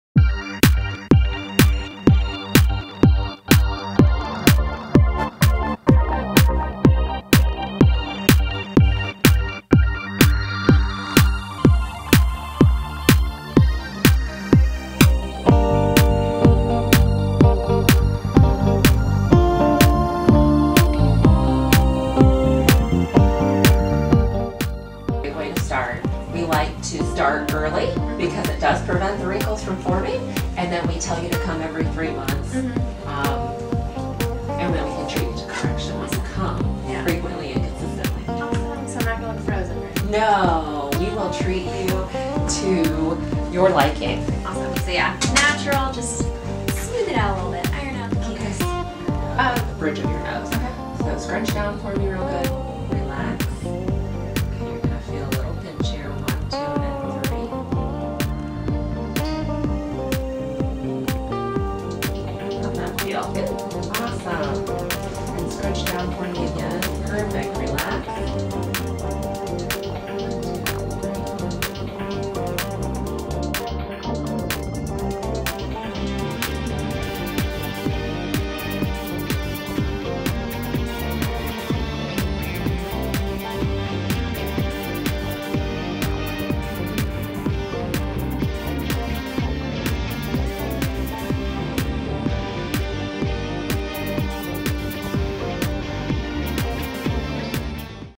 First Time Botox Video
Watch Ellie get Botox for the very first time.
We love that Botox can be used to prevent wrinkles. Talk to your expert provider about creating the perfect plan for your goals.